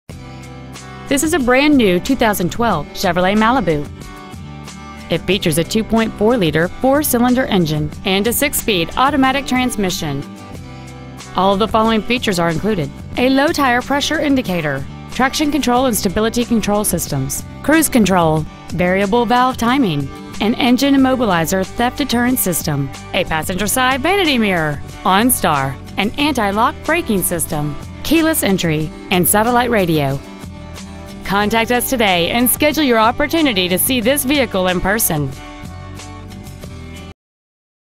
2012 Chevrolet Malibu Gainesville GA 30501
Year : 2012
 Make : Chevrolet
 Model : Malibu
 Engine : Gas 4-Cyl 2.4L/145
 Trans . : Automatic
 Exterior : Black Granite Metallic

 Interior : 83B TITANIUM
 HARDY CHEVROLET INC
 2115 Browns Bridge Road
 Gainesville , GA 30501
 2012 Chevrolet Malibu Gainesville GA